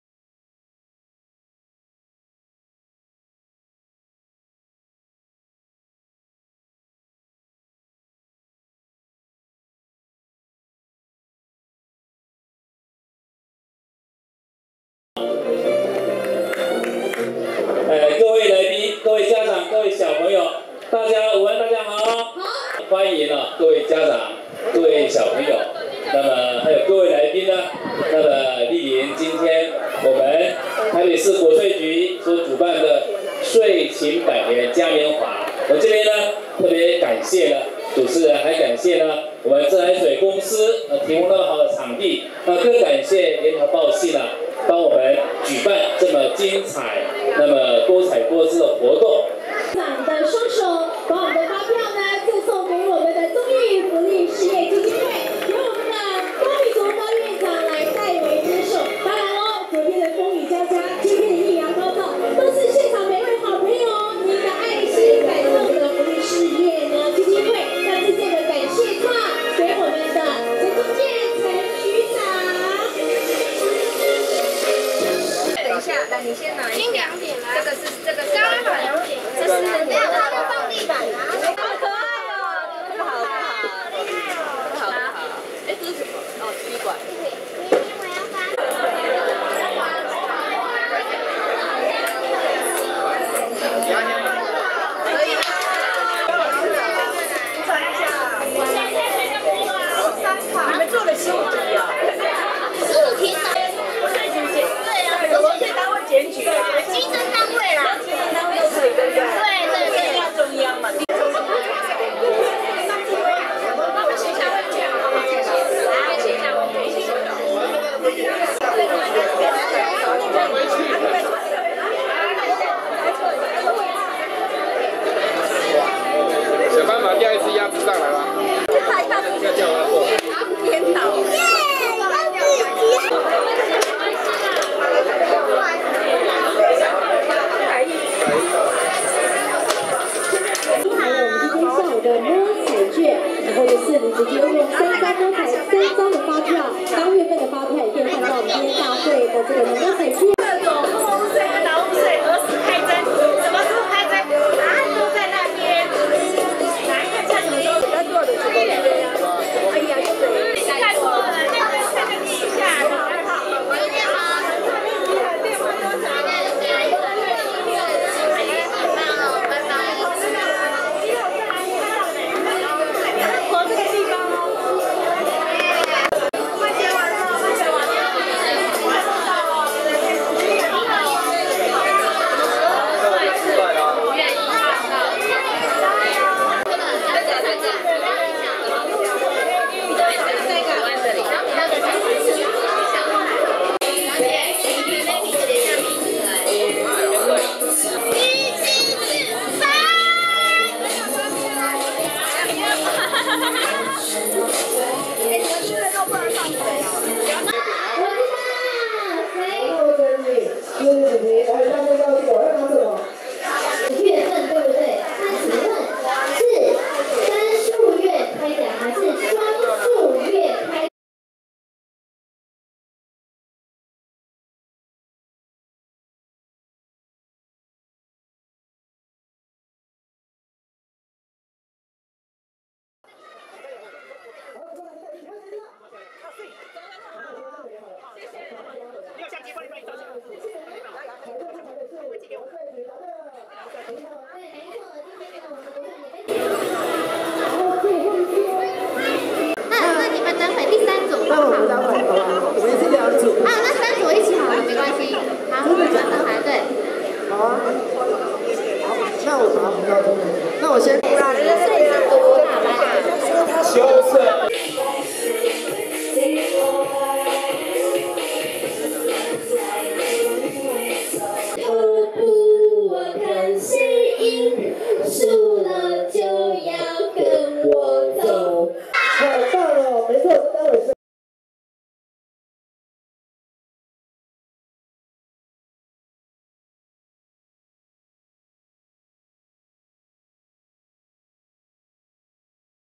稅情百年嘉年華(國稅局指定版)
台北市國稅局舉辦稅情百年嘉年華。